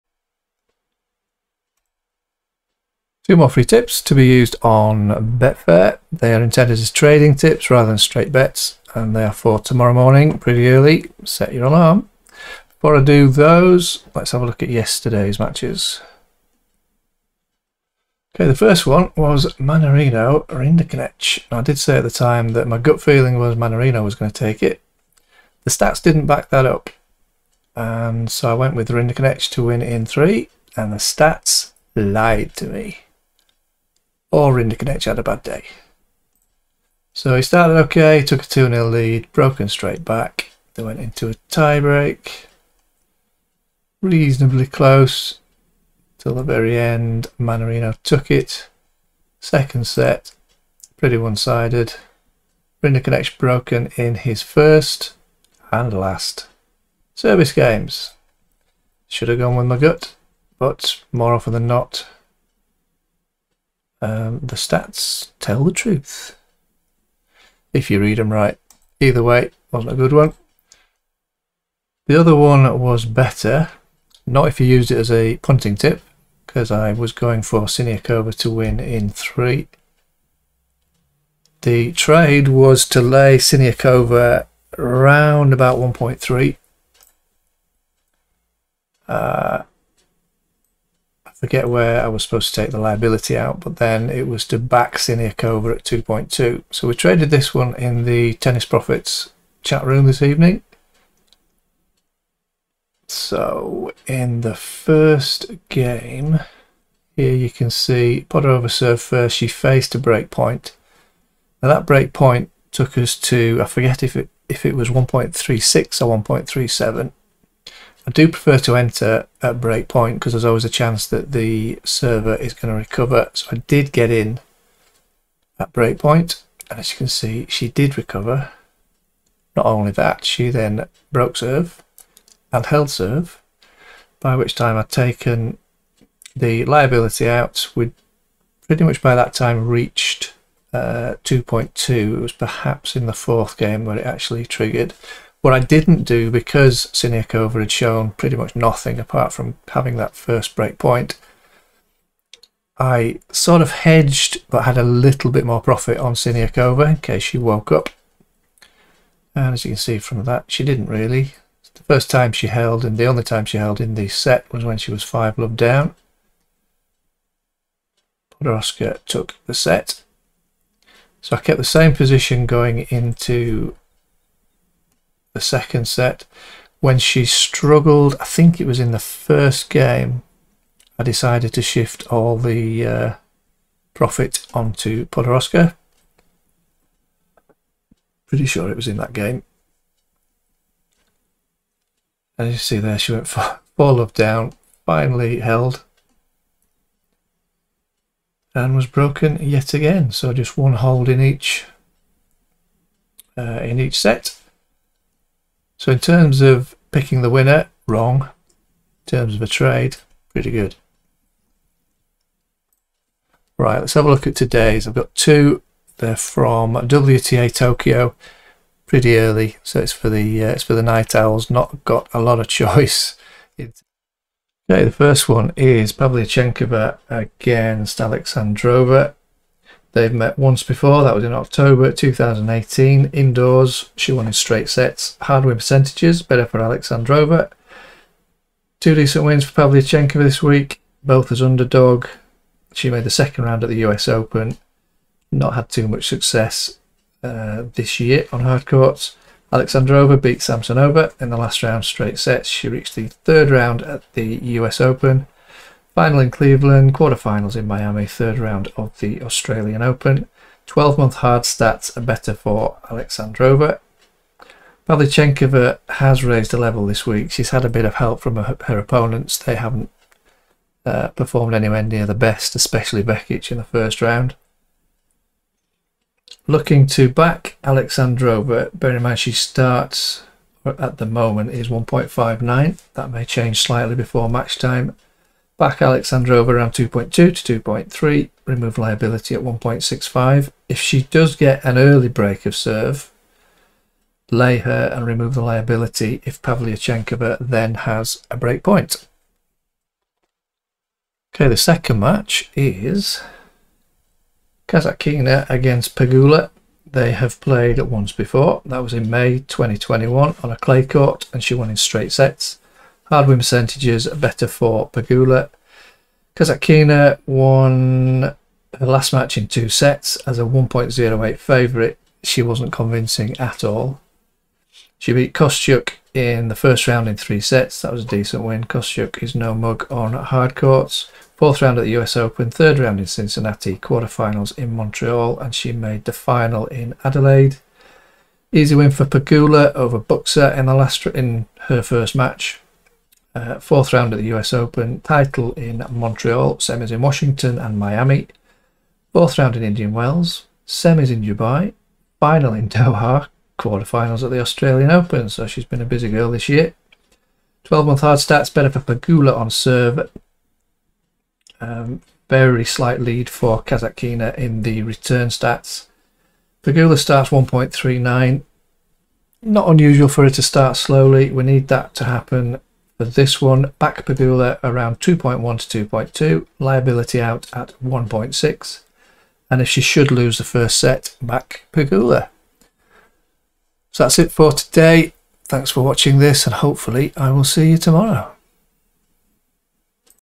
FREE Tennis Trading Tips (For use on Betfair)
2 more FREE trading tips for use on the Betfair match odds markets.
I also review yesterday's matches
If you like these tips sign up to get tips for every ATP and WTA match